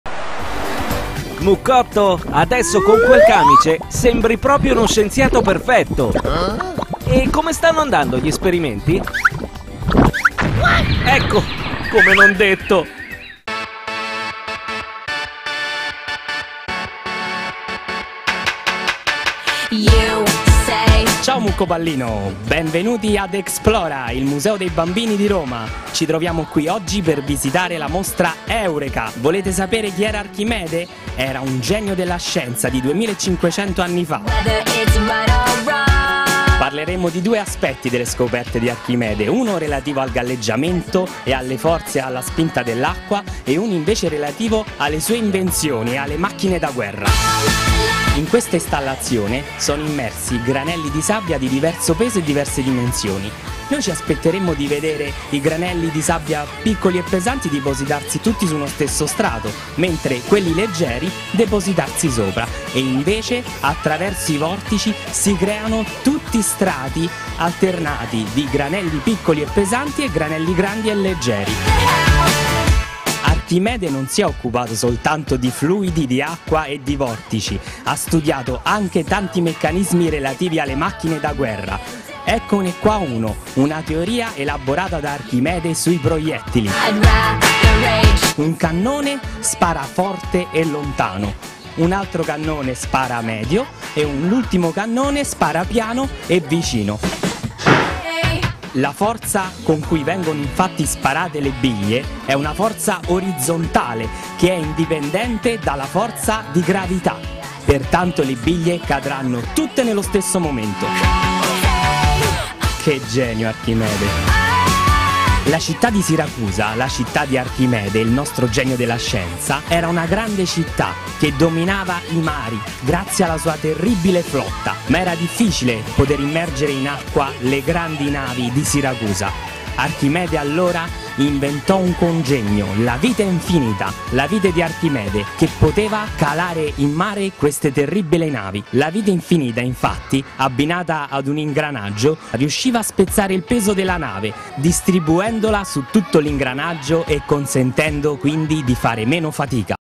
Con Archimede al Museo Explora di Roma
Galleggiamento e macchine da guerra del genio di Siracusa nel museo interattivo dei bambini